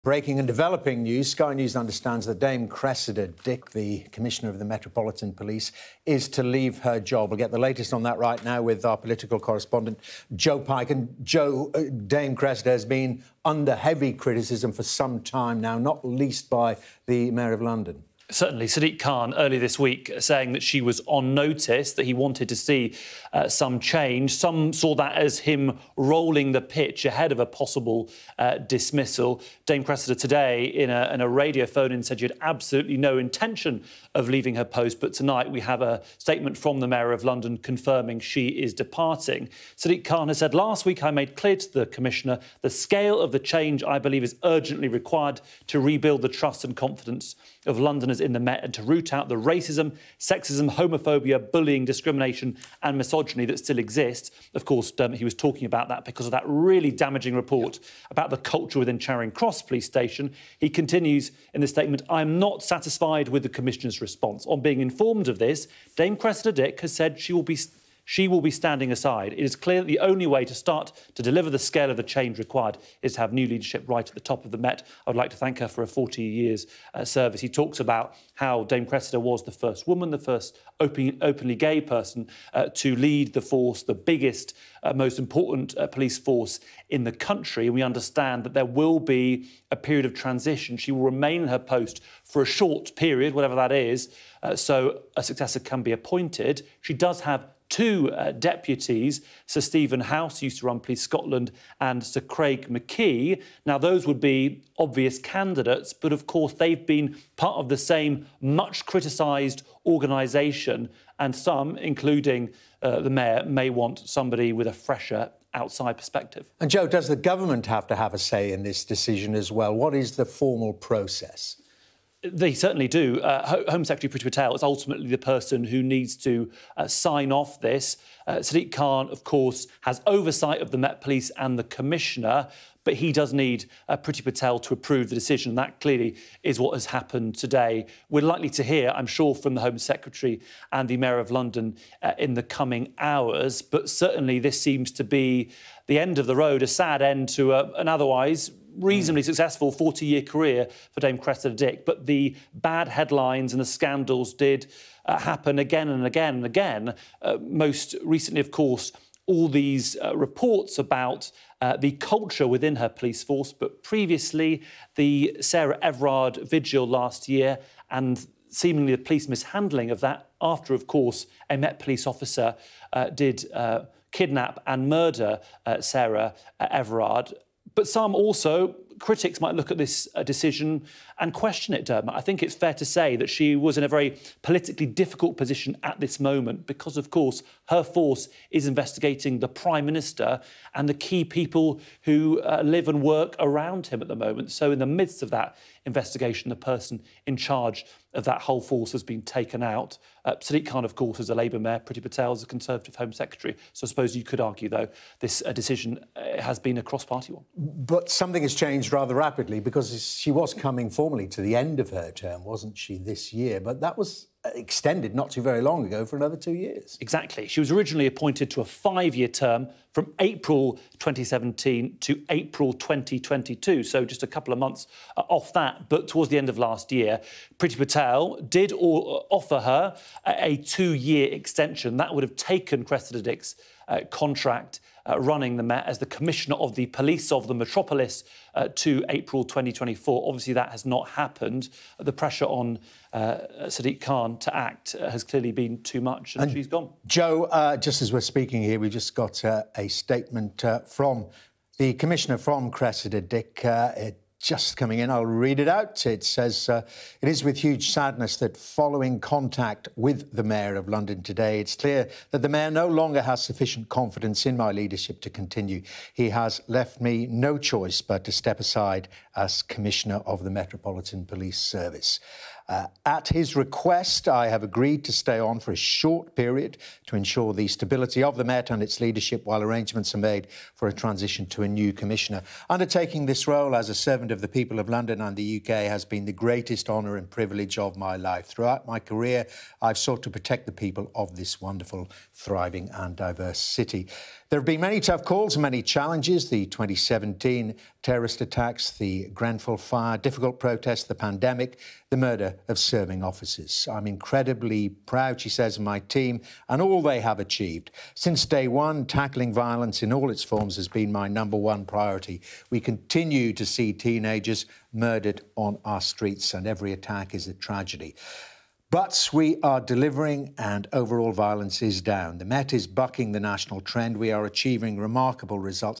Met Police Commissioner Dame Cressida Dick has resigned, Sadiq Khan confirms
Dame Cressida Dick is leaving her role as head of the Met Police, the Mayor of London has confirmed.

Sadiq Khan had put the Metropolitan Commissioner "on notice" last week after the police watchdog published messages sent by officers that used sexist, racist and homophobic language.

He said his entire trust in the Met hinged on her coming up with a robust plan for dealing with the anti-social behaviour.

But he said he was "not satisfied" with the Commissioner's response.